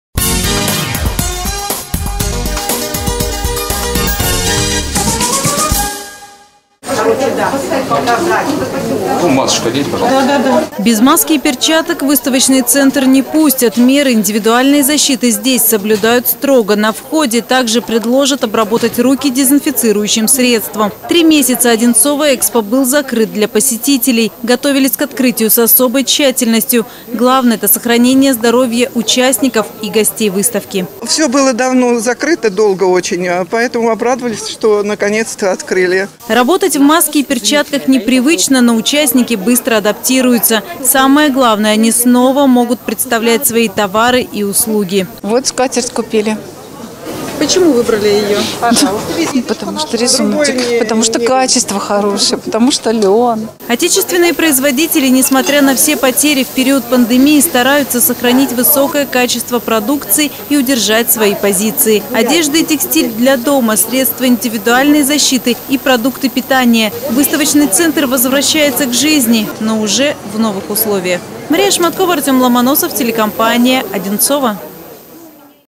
Летняя мозаика
Маски, перчатки, санитайзеры, соблюдение дистанции. В «Одинцово-Экспо» открылась выставка «Летняя мозаика». Участники из разных регионов приехали в наш округ, чтобы представить свою лучшую продукцию. Как работает выставочный центр в новых условиях?